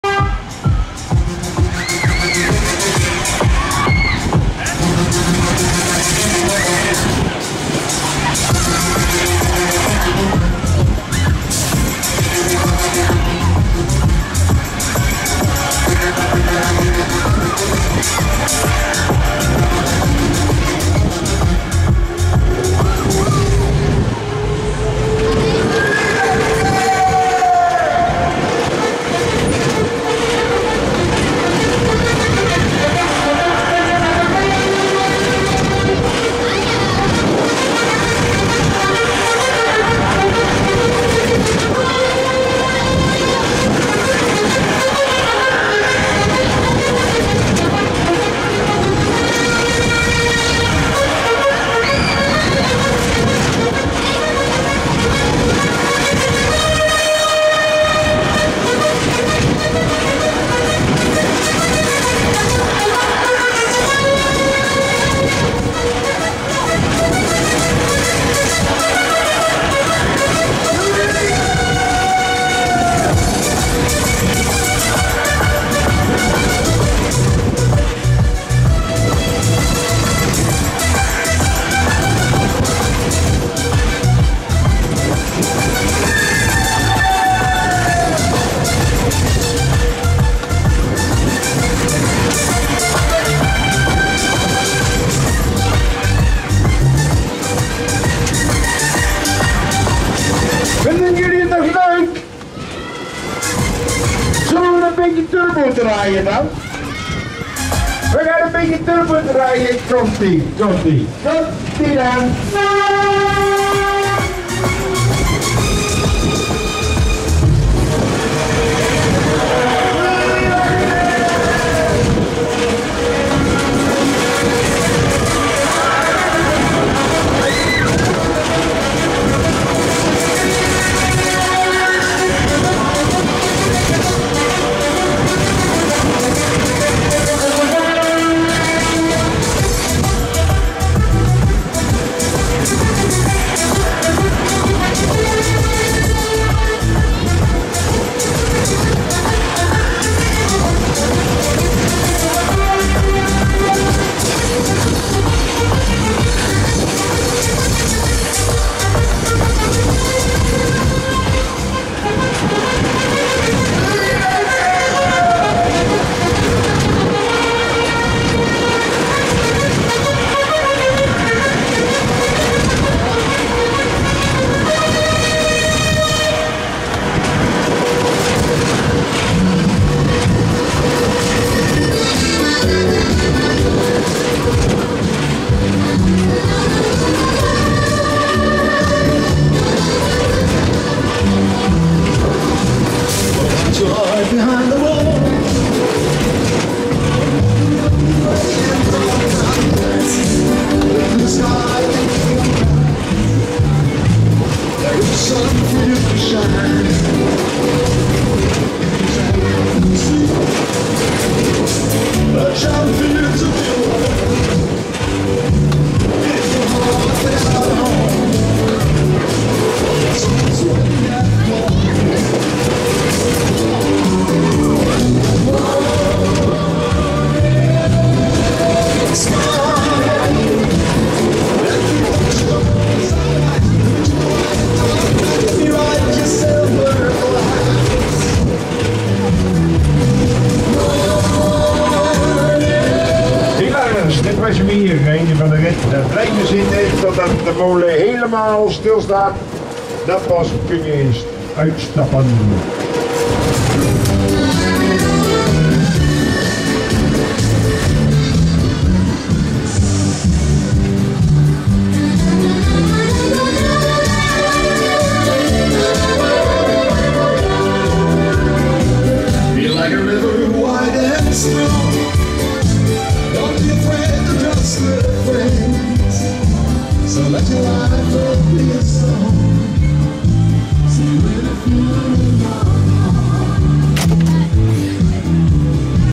Calypso (Kroon-Hoefnagels) - Innenfahrt/Onride [Eindhoven (NL) - Park Hilaria 2017]
Calypso vom Schaustellerbetrieb Kroon-Hoefnagels (NL) auf dem Park Hilaria in Eindhoven (NL) 2017.

Zig Zag Marke Safeco